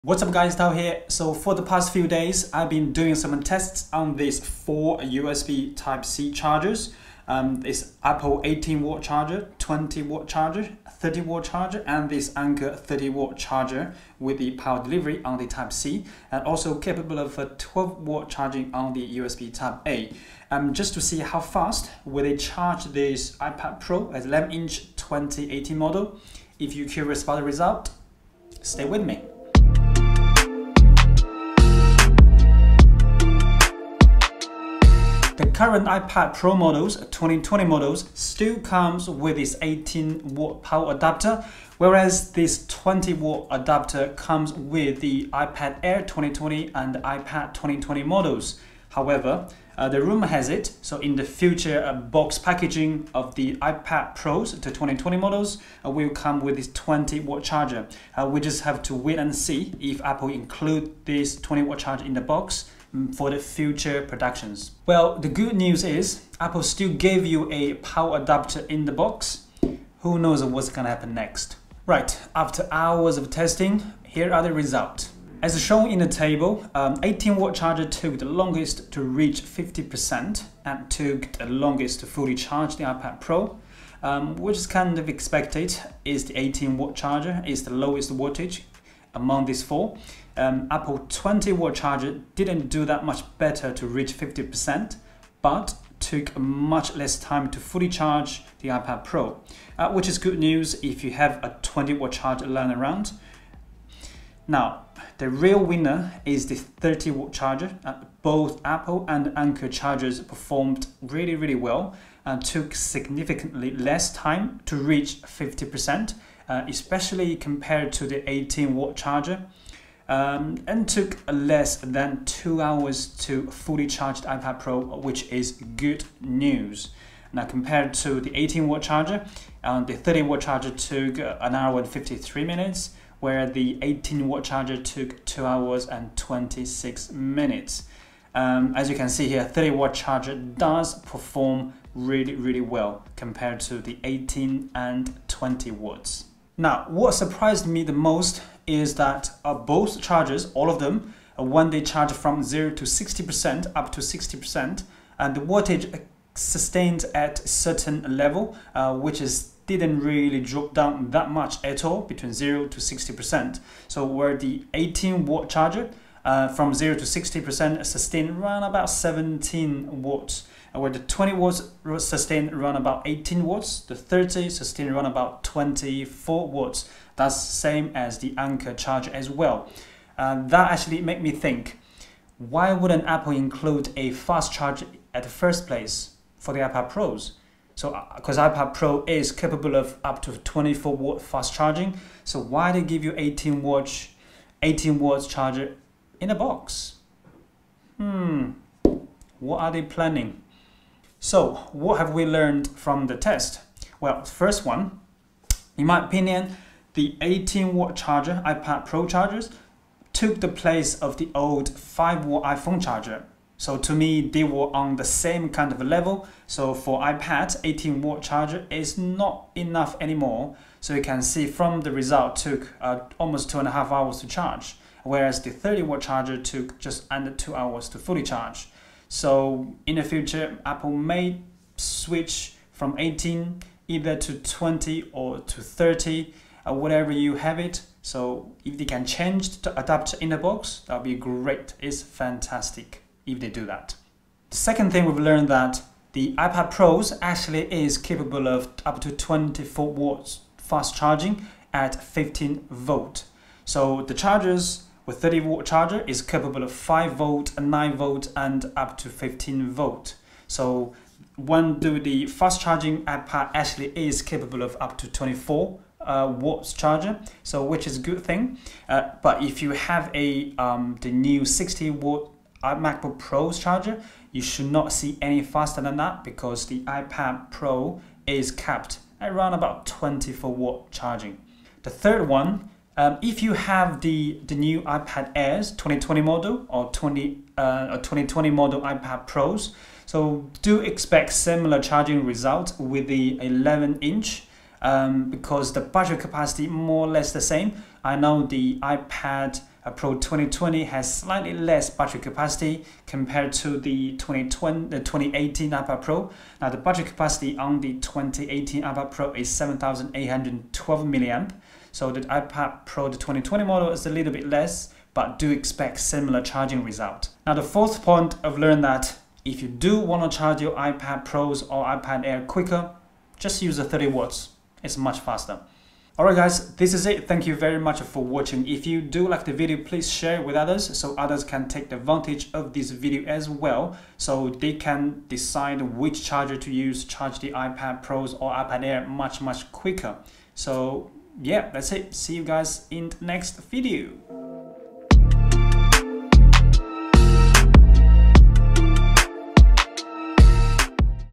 How to charge iPad Pro EVEN FASTER? 18W, 20W,30W
Which charger should you use to charge your iPad Pro?
In this video, we will find out which charger charges ipad pro the fastest. 18W, 20W or 30W?

Apple 20W charger